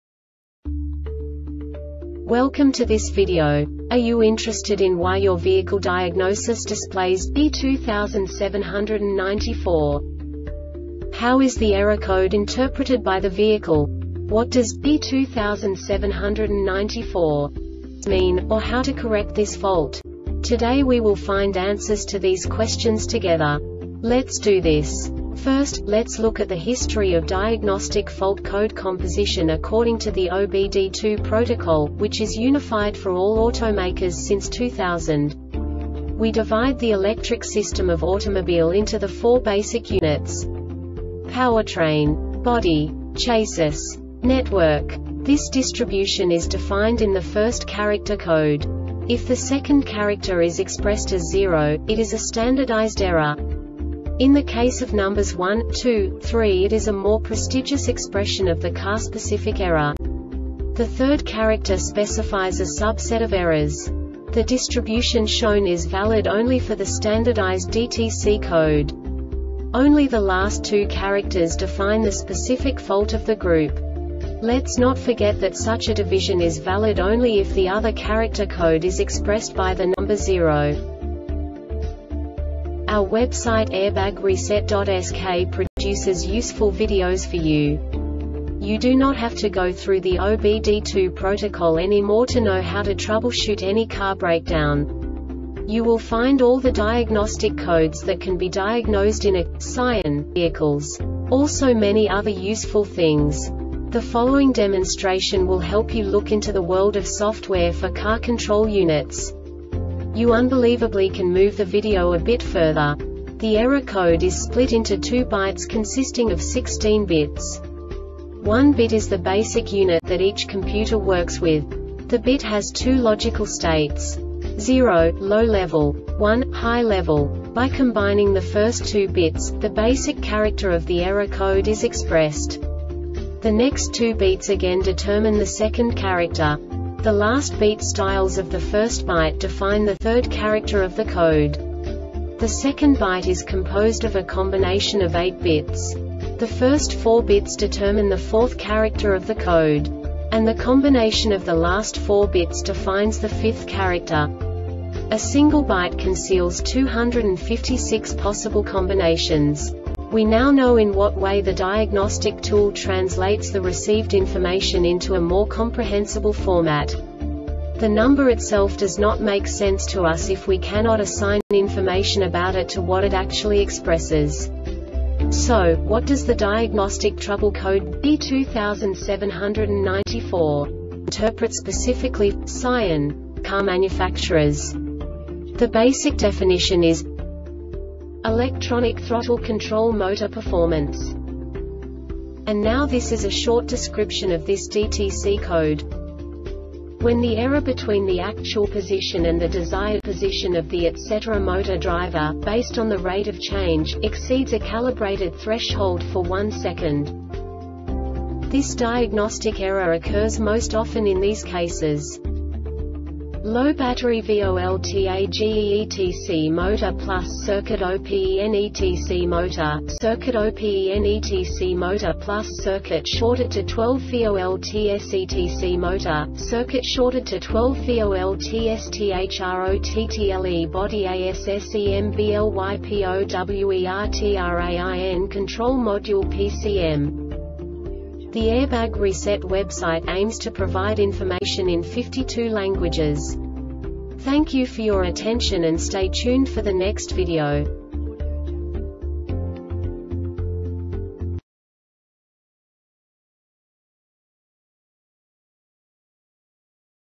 DTC Scion B2794 Short Explanation
Contents:
0:21 Basic DTC analysis according to OBD2 protocol standard.
1:48 Insight into programming
2:58 A diagnostically understandable description of the Diagnostic Trouble Code
3:05 Basic error definition

Make:
Scion
Code:
B2794
Definition:
ELECTRONIC THROTTLE CONTROL MOTOR PERFORMANCE
Description:
When the error between the actual position and the desired position of the ETC Motor Driver, based on the rate of change, exceeds a calibrated threshold for one second.
Cause:
LOW BATTERY VOLTAGEETC MOTOR (+) CIRCUIT OPENETC MOTOR (-) CIRCUIT OPENETC MOTOR (+) CIRCUIT SHORTED TO 12 VOLTSETC MOTOR (-) CIRCUIT SHORTED TO 12 VOLTSTHROTTLE BODY ASSEMBLYPOWERTRAIN CONTROL MODULE (PCM)